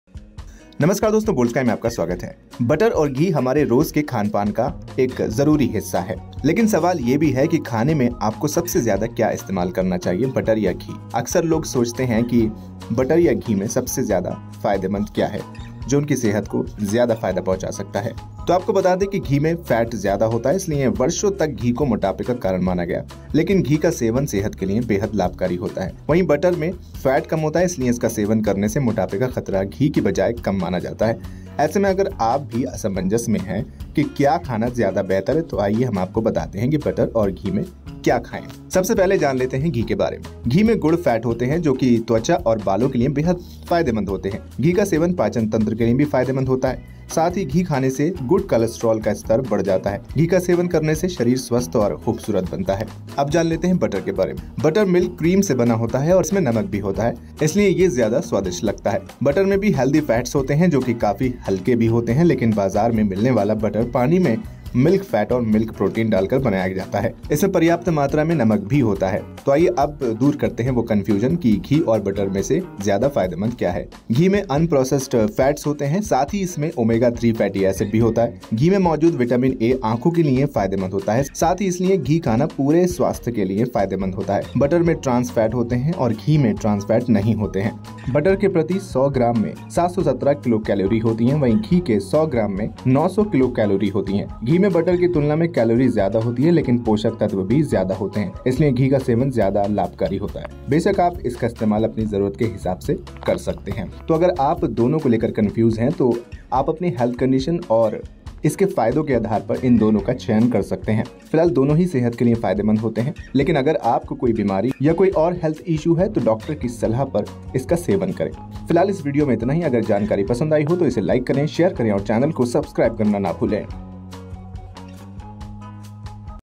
Butter Or Ghee सेहत के लिए क्या सबसे ज़्यादा फायदेमंद | Boldsky *health
घी और मक्‍खन दोनों ही फायदेमंद होते हैं। आइए जानते है घी और मक्‍खन के बीच क्या अंतर है और दोनों में से अधिक हेल्दी कौन है?

Ghee and butter both are beneficial. Let us know what is the difference between ghee and butter and which is healthier?